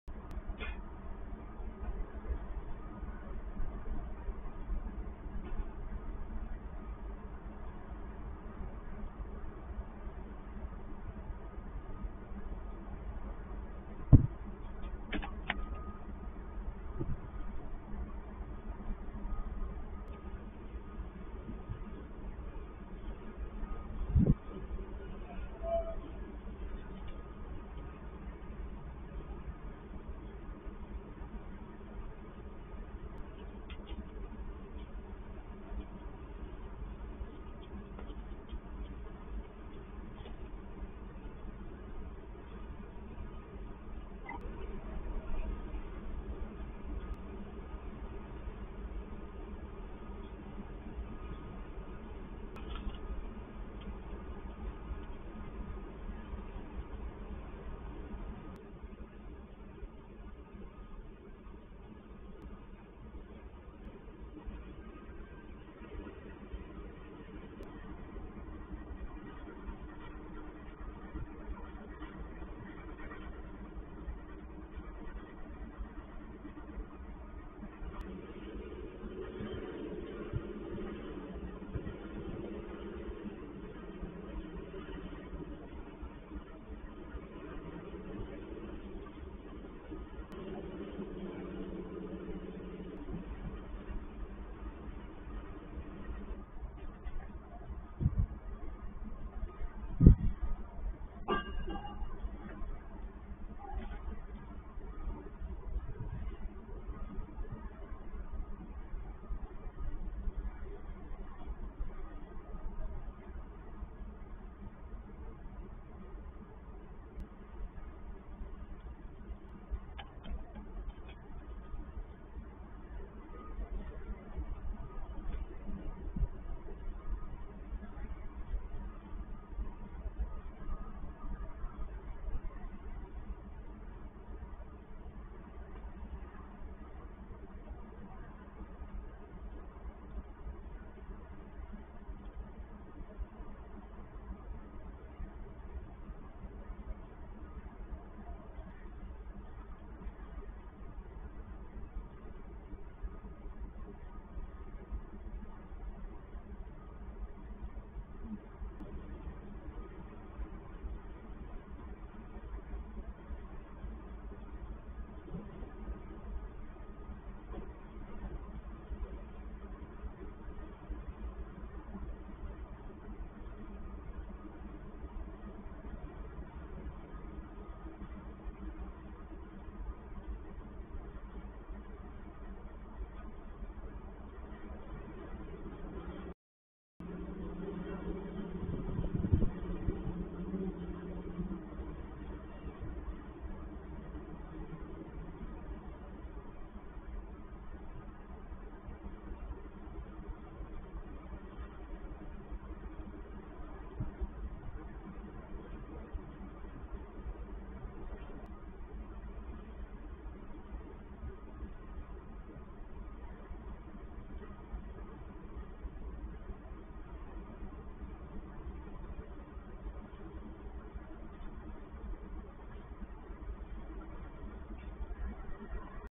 CEZ Skawina PL -Yangster 5CD for an hour was cleaning the feathers in the doorway-2021 11 29 08 02
Yangster was not hungry, he did not touch the remnants, he flew away